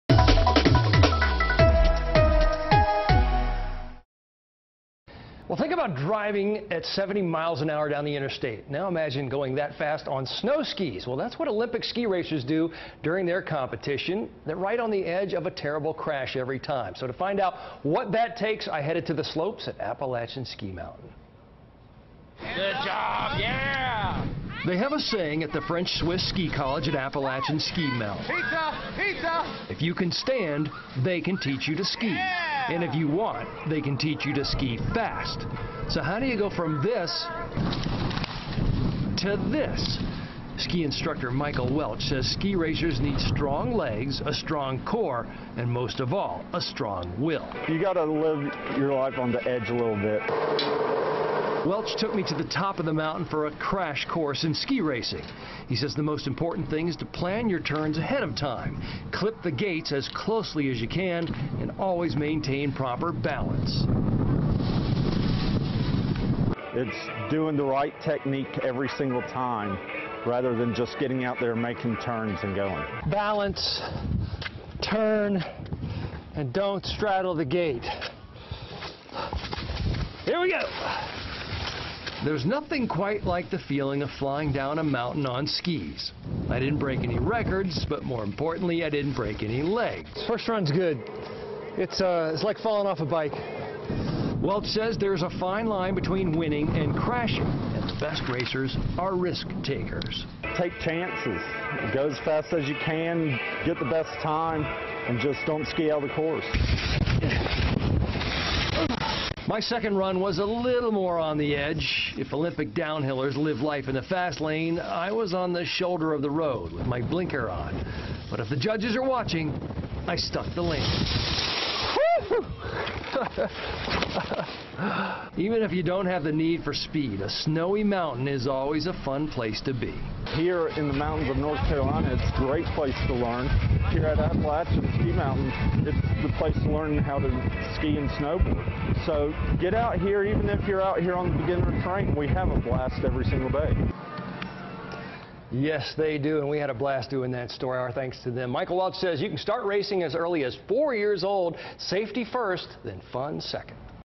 Cameron tries Olympic ski racing!
WXII's Cameron Kent hits the slopes at Appalachian Ski Mountain and learns what it takes to tackle ski racing.